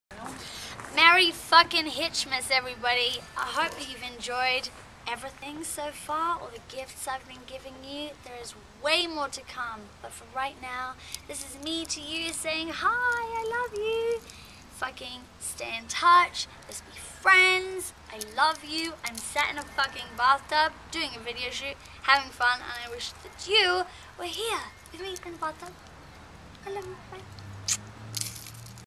Merry Hitchmas From Neon Hitch! [Vlog]
Merry Hitchmas From Neon Hitch! Collect your gift including new music, new video, and new photos on Facebook! Like me because I love you!